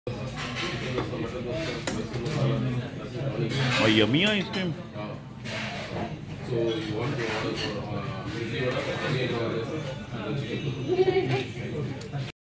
Ice Cream Lover
Having icecream after dinner :) Yummmy